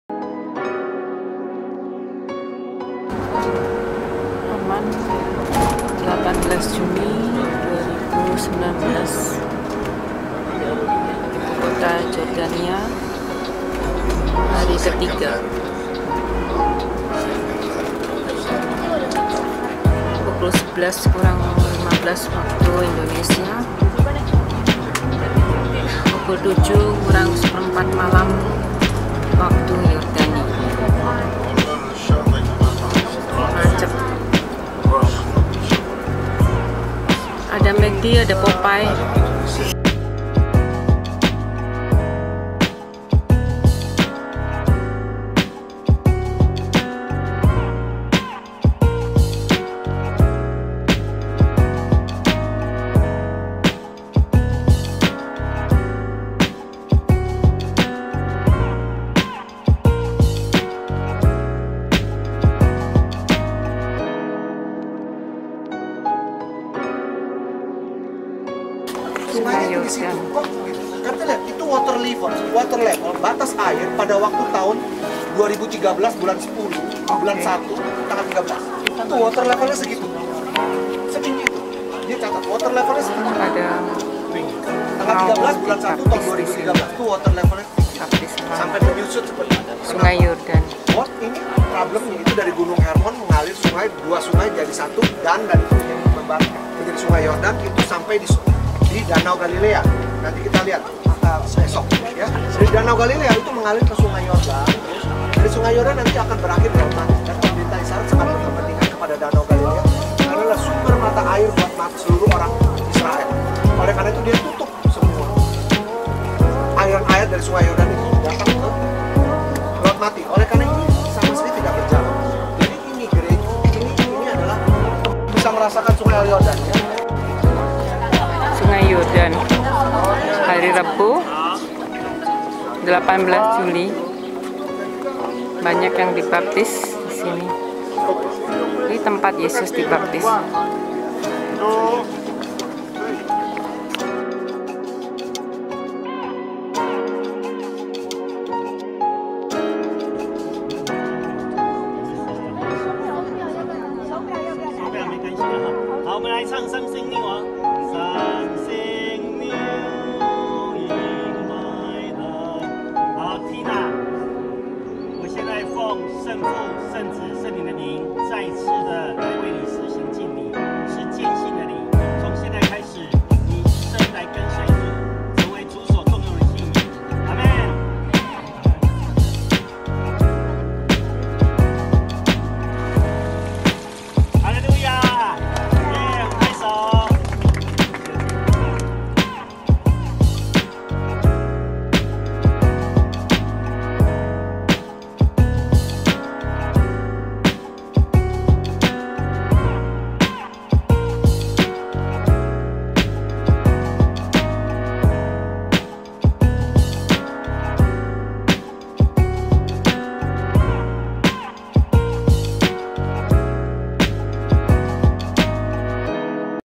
QASR EL YAHUD-JORDAN RIVER PART#1-THE WEST BANK JORDAN RIVER
QASR EL YAHUD- THE PLACE WHERE JESUS WAS BAPTIZED BY JOHN THE BAPTIST -  ON JORDAN RIVER PART #1- THE WEST BANK JORDAN RIVER

Journey of Joy- Sharing For Goodness- Joana Keyrupan

Pagi ini kami menyebrangi border dari Jordan-Israel lewat King Husein Bridge bagian dari Jericho-Palestine, menuju Qasr El Yahud tempat dimana Yesus dibaptis oleh Yohanes pembaptis, sebelum Yesus memulai karya Nya  tampil depan umum, dan sebagai simbol kelahiran secara spiritual.

Tempat alternatif Baptismal site lainya adalah Yardenit,  lokasinya berada di utara Israel dekat selatan Galilea. Yardenit akan kami kunjungi besok untuk pembaptisan dan pembaharuan janji baptis setelah misa di Mount Tabor , di Yardenit  airnya lebih jernih dan dekat dengan Danau Galilea.

 Sungguh pengalaman yang menarik dapat menapak tilas menyusuri tempat-tempat dimana Yesus pernah hidup di tanah terjanji, Galilea dan Yerusalem.

Next video is Dead Sea (Laut Mati ) is the west dryland in the world- 1360 ft (414 m) in Israel, Jordan, and Syria.

credit: baptism video by Indira, Ferdian